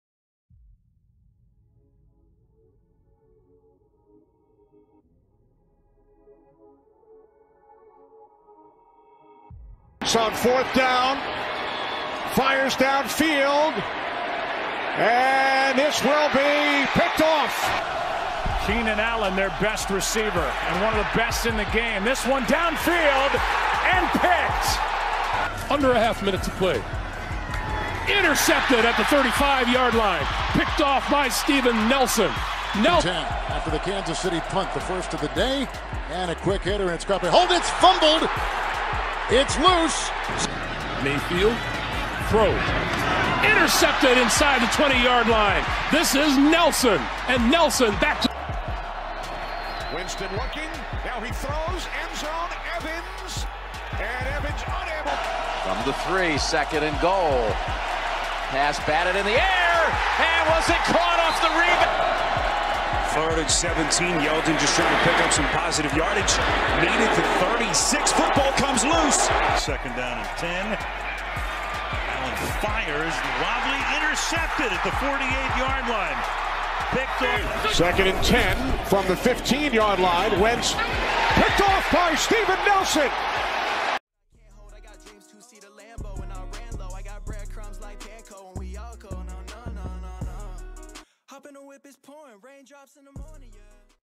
HOUSTON TEXANS: Steven Nelson ᴴᴰ
BREAKING NEWS || HOUSTON TEXANS || STEVEN NELSON HIGHLIGHTS
98 CAREER GAMES
341 TACKLES
8 INTERCEPTIONS
1 FORCED FUMBLE